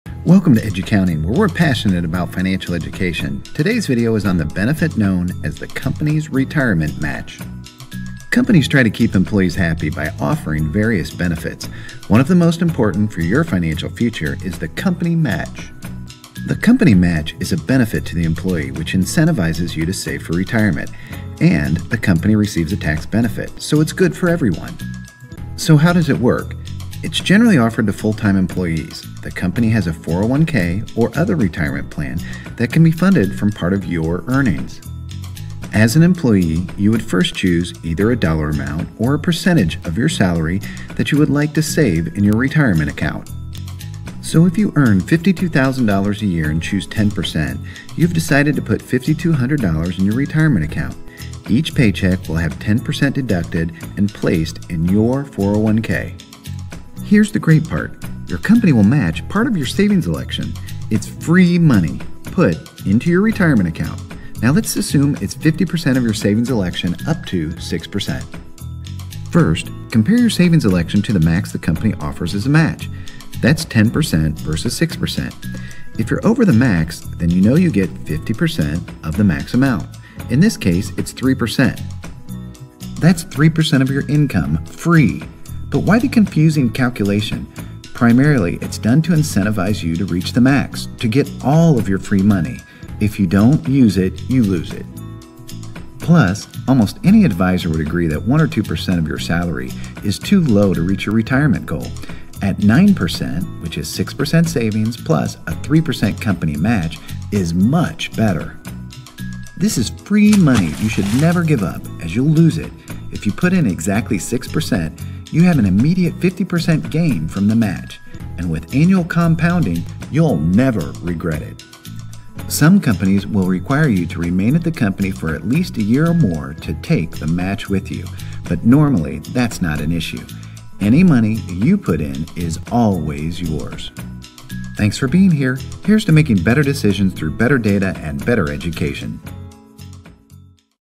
What is a Company Retirement Match?
What is a company retirement match and does your company offer it for retirement? It's important to find out because if they do, this is FREE money. Find out how investing in yourself and having your job invest in you can help you secure your retirement.